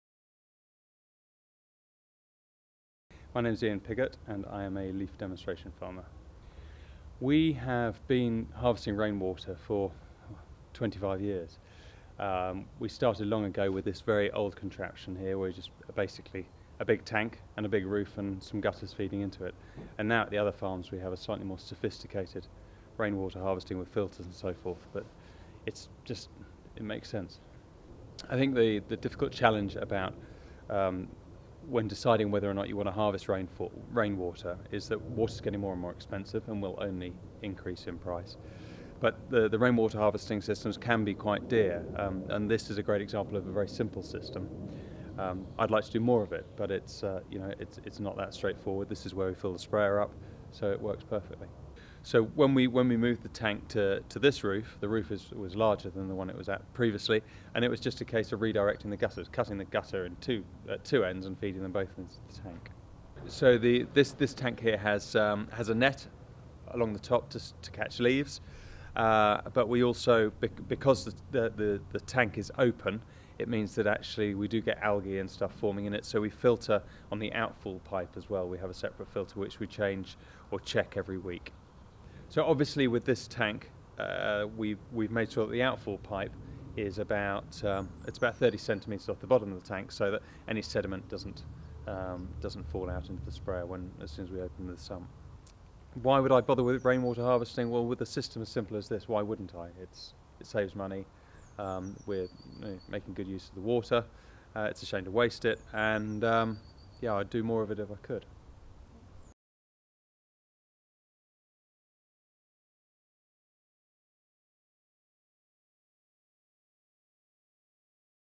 A Guide to Rainwater Harvesting on Farm
In this video, LEAF Demonstration Farmer, Ian Pigott, explains how he has set up a simple rainwater harvesting system. Ian explains how the water is used and a few basic principles to ensure the system is kept in good order.